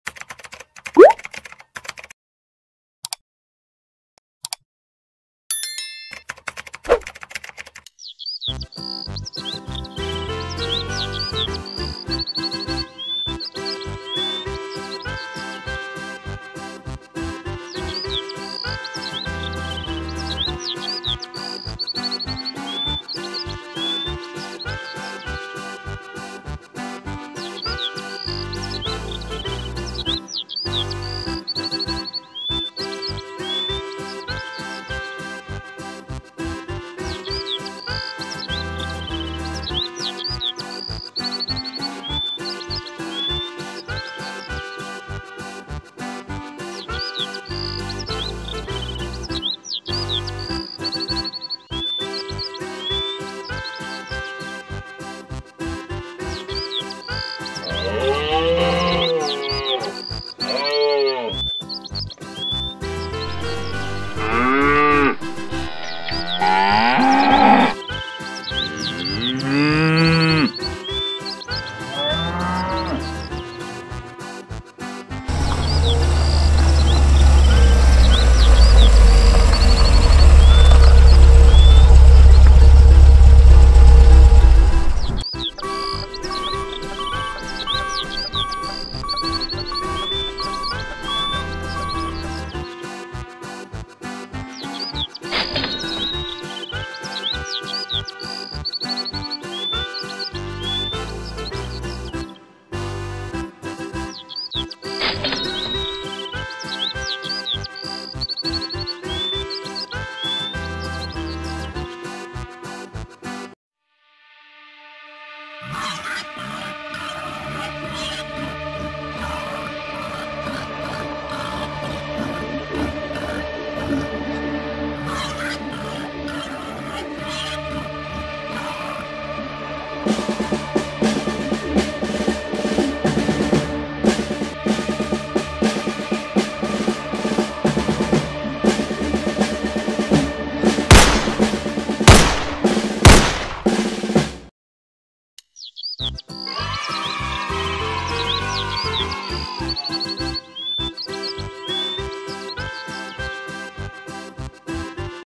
Happy farmer in beautiful small town
Happy farmer in beautiful small town. Wide green fields, flowing rivers, high mountains, beautiful nature. The farms are healthy, and the mountain atmosphere is very peaceful, but there are times when wild boars destroy plantations, and farmers have to guard and drive away the wild boars